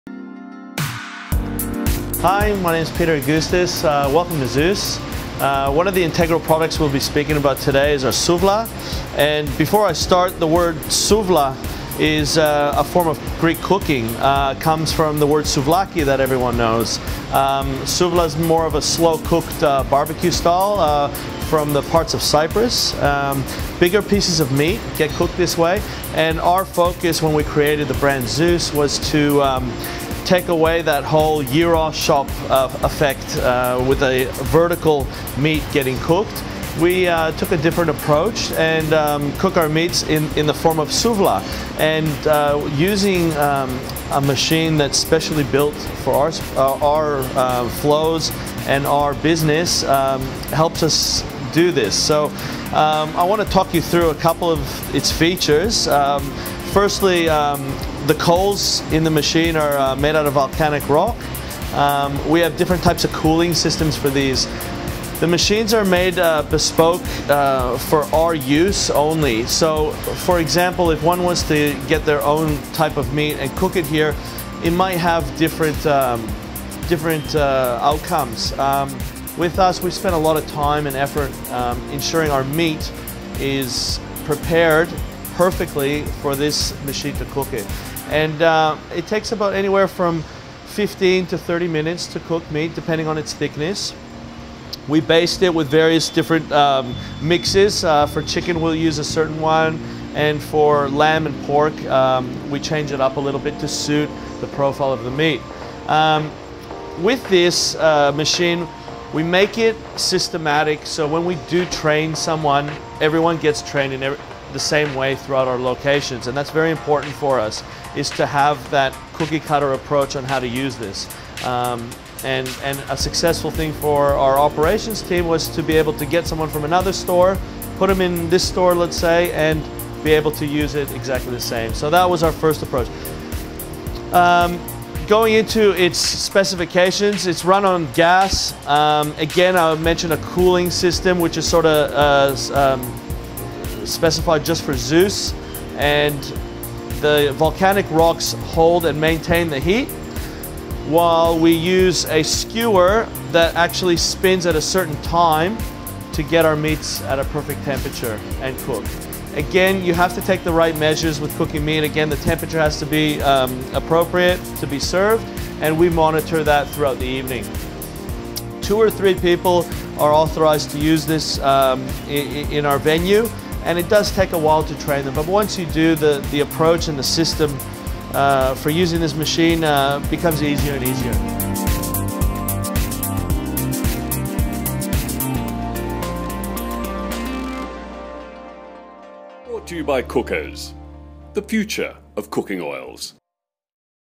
Kitchen Tour: Zeus Street Greek
Check out Zeus Street Greek's custom made souvla machine, which is gas operated and uses volcanic rock to maintain heat.